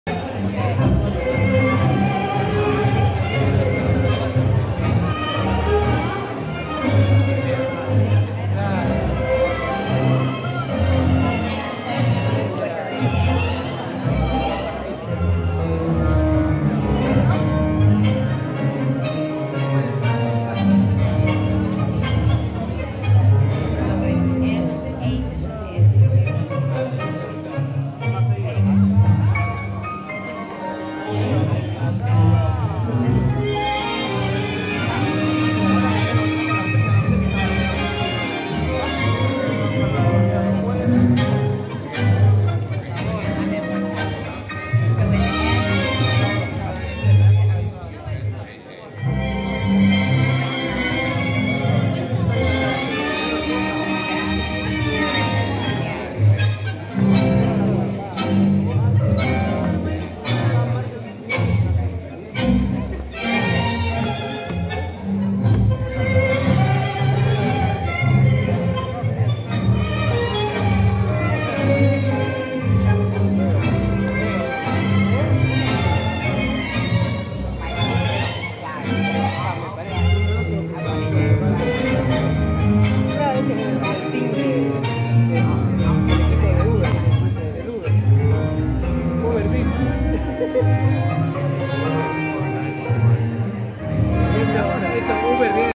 Tango dancing on milonga dancing floor, Buenos Aires
Please follow @AiresDeMilonga has the tango video library from Buenos Aires, tango cradle, you can support it